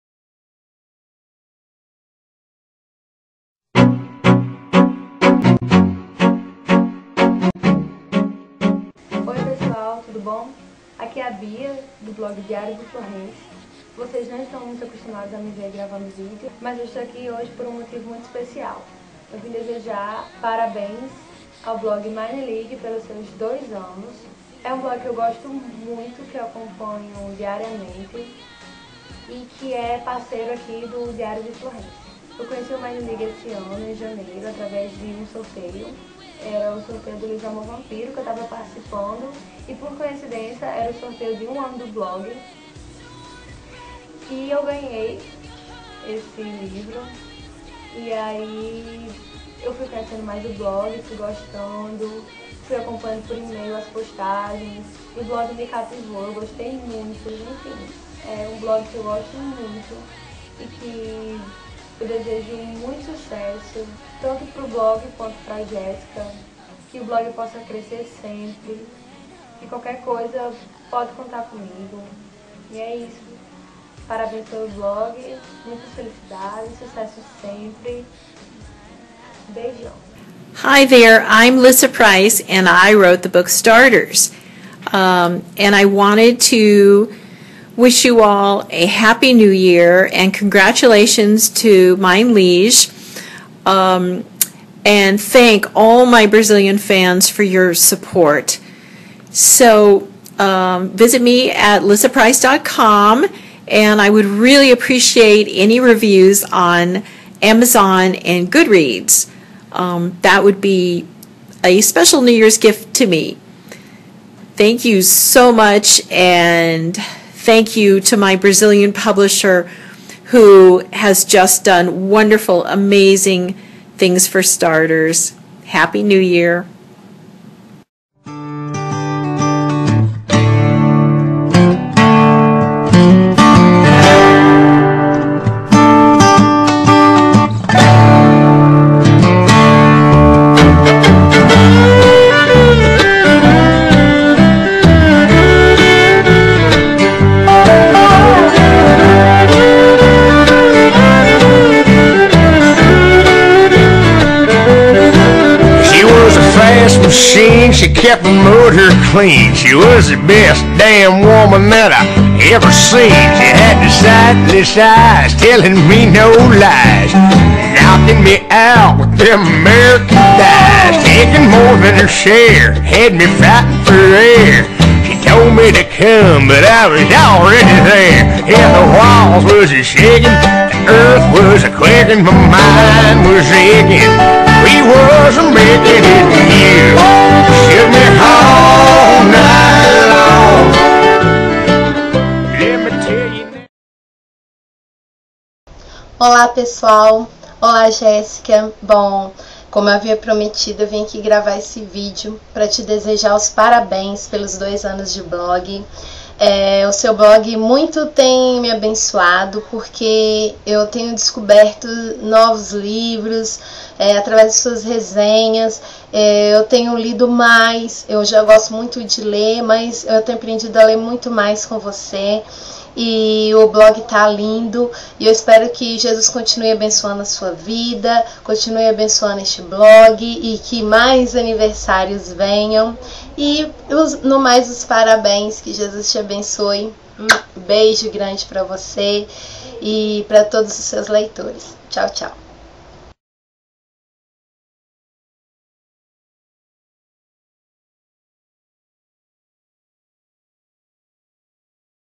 Dois anos Meine Liege!
Olá galera!!

Hoje, dia 01.01.13, desejo a vocês um ano maravilhoso, cheio de conquistas, bençãos e determinação! Que haja alegria e perseverança nos momentos difíceis!!

E, claro, o Meine Liege completa dois anos de existência!! Comemoremos!!!
\o

Está rolando uma super festa com uma galera maravilhosa no blog, confira no post

Para minha surpresa e felicidade, outra galerinha gravou um vídeo dando parabéns para o blog! A essas pessoas um beijo super especial: Bia Medeiros, Lissa Price, Fabian Balbinot e Daniela Rodrigues!

Um beijo para todos! Pegue o seu pedaço de bolo e seu refri , vamos comemorar!!

P.S: Em breve, promoção especial de aniversário! ;*